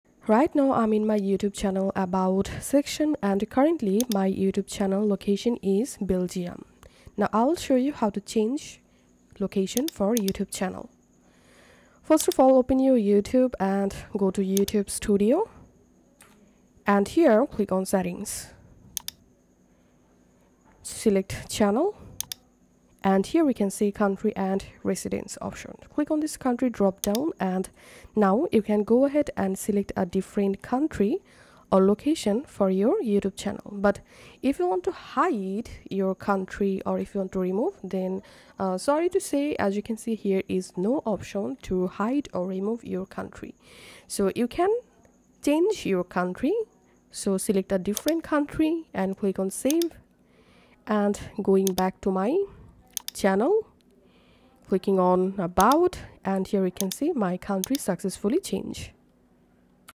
How to hide or change country location on YouTube - Full Guide
YouTube suggests videos to you based not only on your preferences but also on your location. However, you can easily change your default location on YouTube to receive personalized suggestions for other countries. see on to learn how to change your location on YouTube.